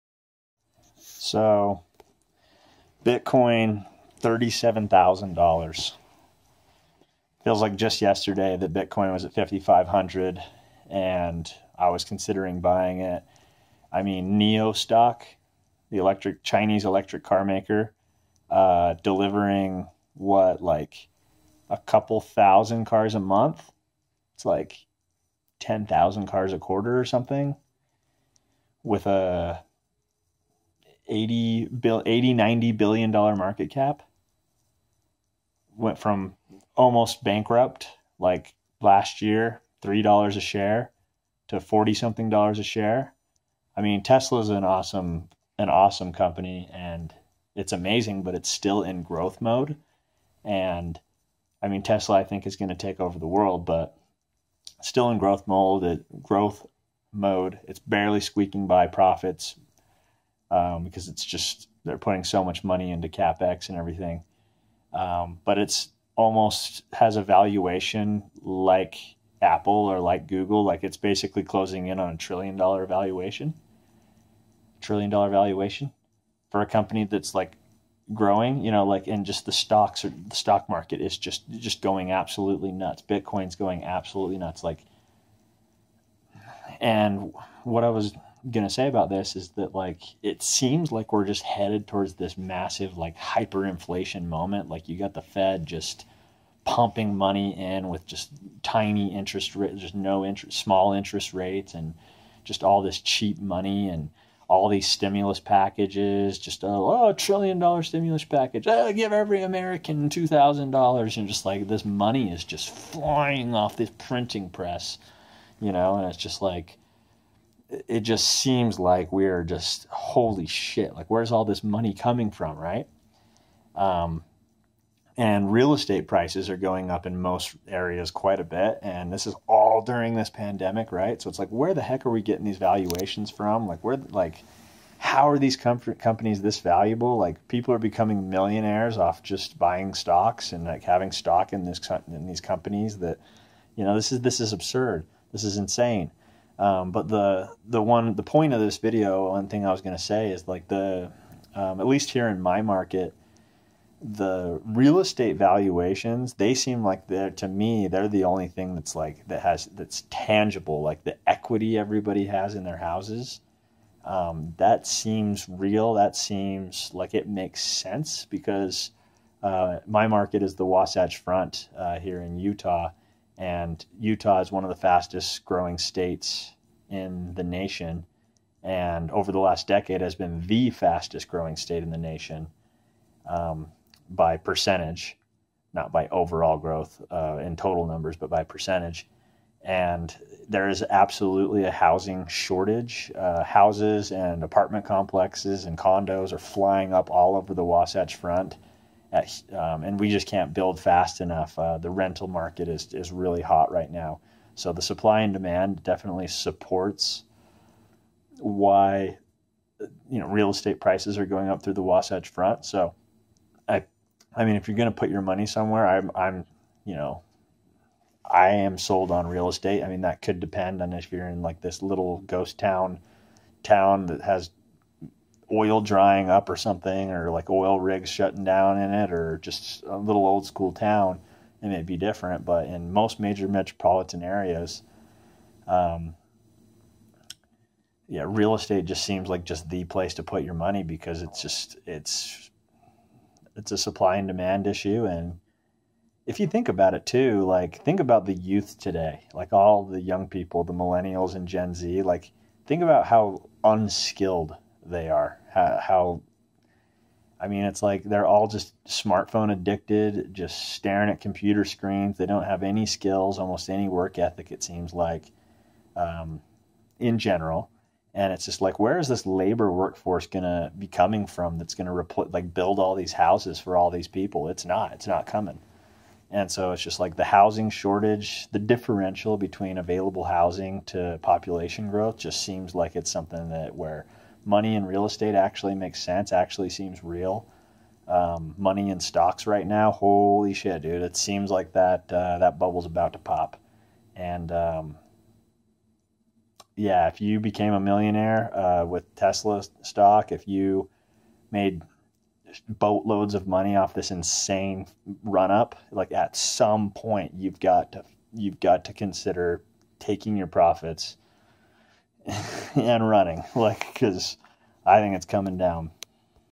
Bitcoin 40k? Is this even real? Tesla nearly a trillion dollars? Nio $45 a share?
Seems like this run has to come to an end at some point...